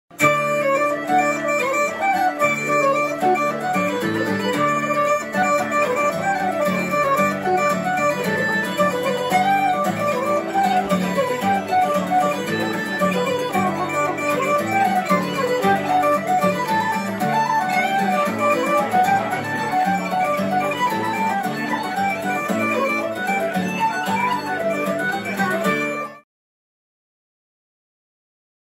Cillian Vallely & Kevin Crawford
For Revelry’s return to the theater, Cillian Vallely, Kevin Crawford, and Allan Murray turn our stage into your new local pub. While you’ll have to drink your Guinness before you arrive, join us in celebrating the end of the week with lively Irish music. (This event has passed.)